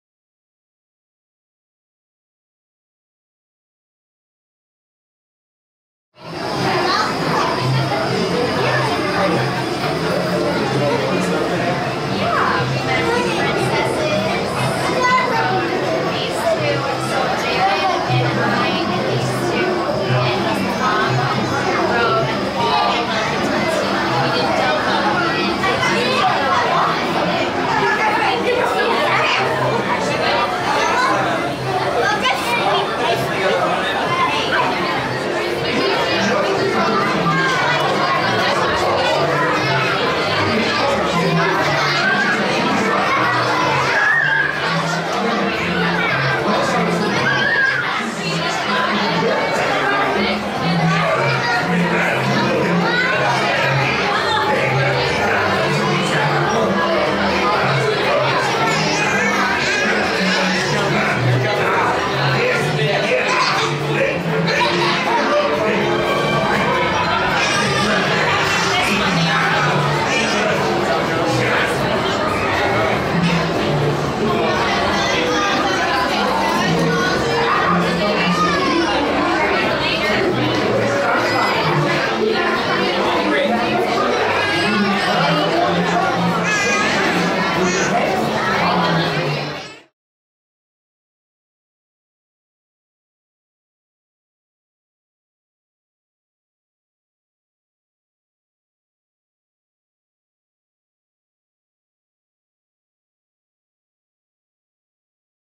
NEW Seven Dwarfs Mine Train Roller Coaster Pre-Show Video - The Magic Kingdom, Walt Disney World
A new video screen has been added to the Seven Dwarfs Mine Train attraction Fantasyland in hopes of speeding up the boarding process.